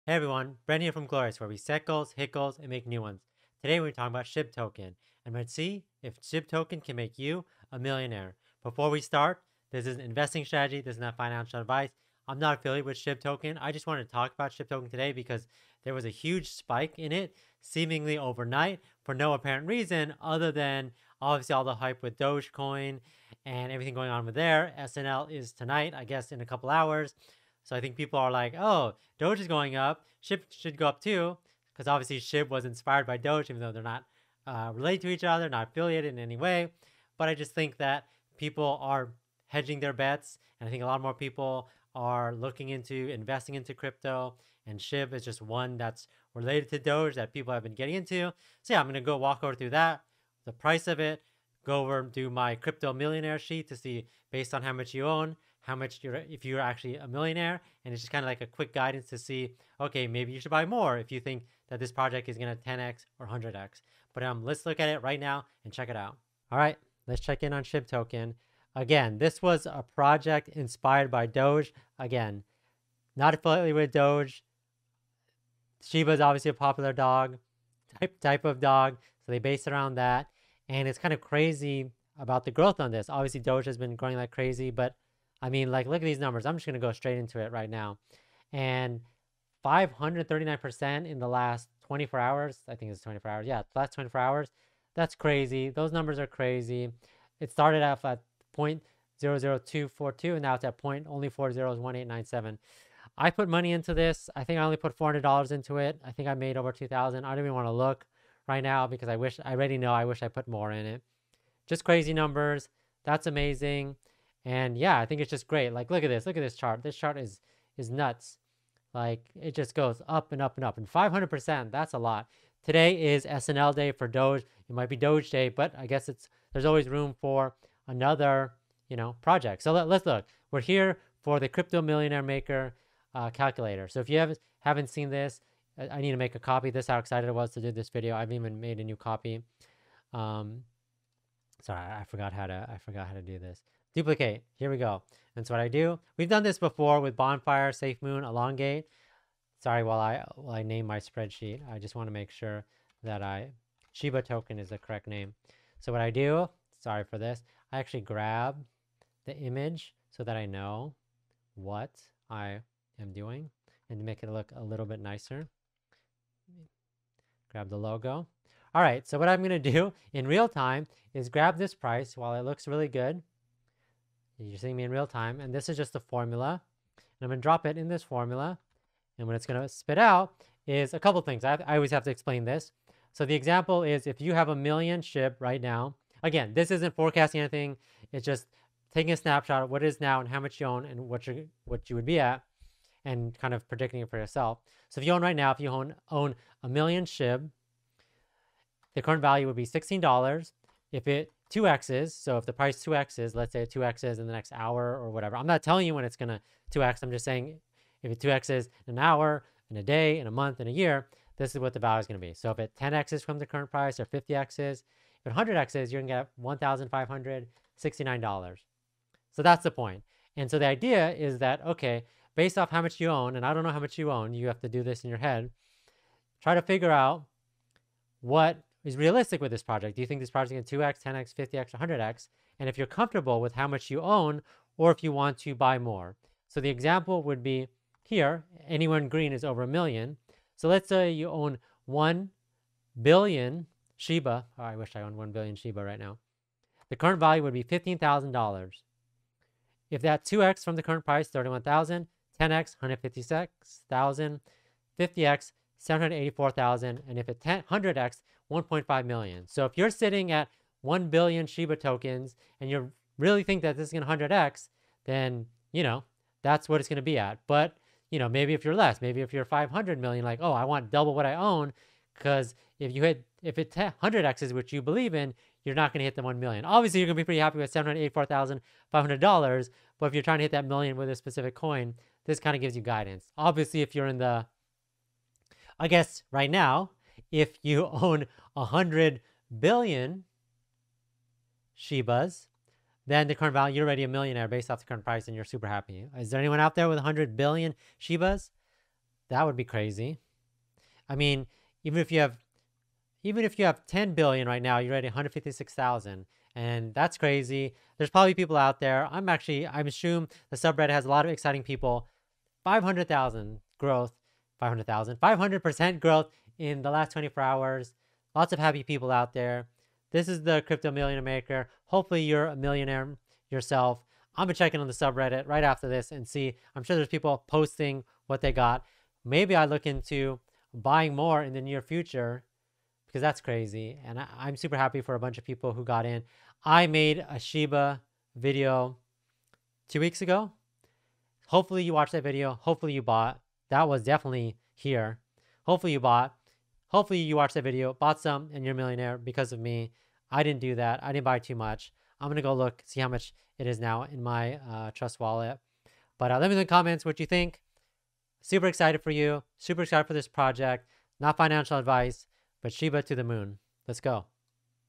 SHIBA INU TOKEN UP 500% - CAN SHIB MAKE YOU A MILLIONAIRE? | Shiba Inu Coin (SHIB)
Wow just wow! Amazing growth for this token is just a short amount of time. Hopefully you saw my last video because this just had a major PUMP. Come check it out!